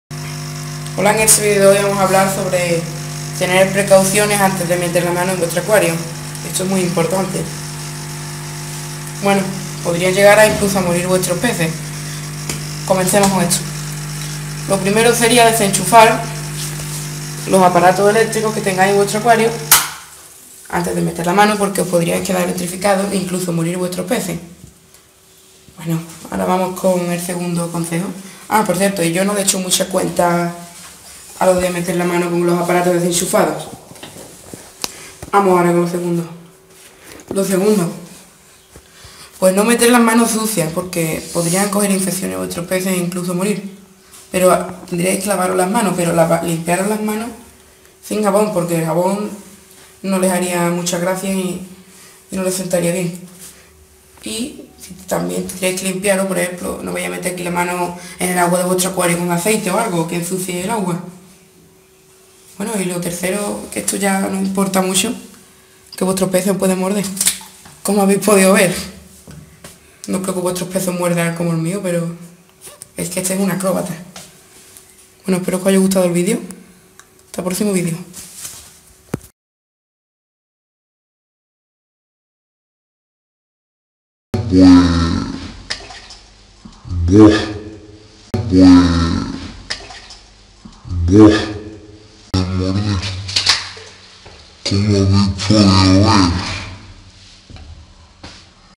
Precaucion antes de meter la mano en el acuario
tambien meter la mano estresa a los peces sobre todo hacer movimientos bruscos